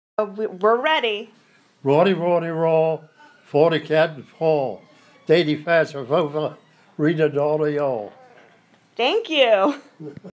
4:15:56 PM
My grandfather reciting a Pennsylvania Dutch speech his grandfather taught to him as a child.